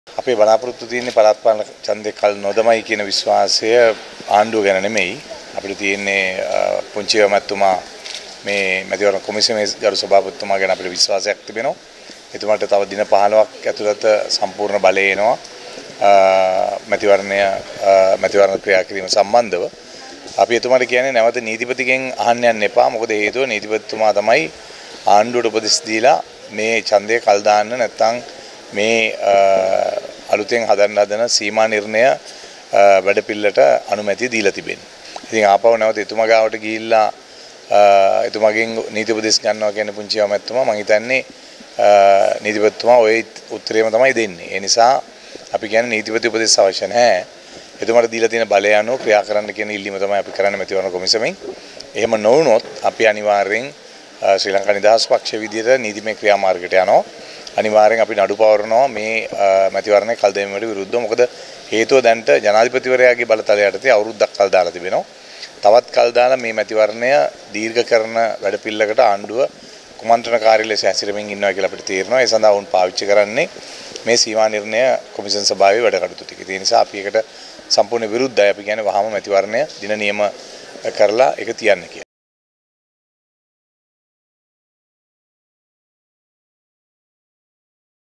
මැතිවරණය කල් දැමීමට විරුද්දව අපි නඩු පවරනවා | Breaking.lk
මැතිවරණය කල් දැමීමට විරුද්දව අපි නඩු පවරනවා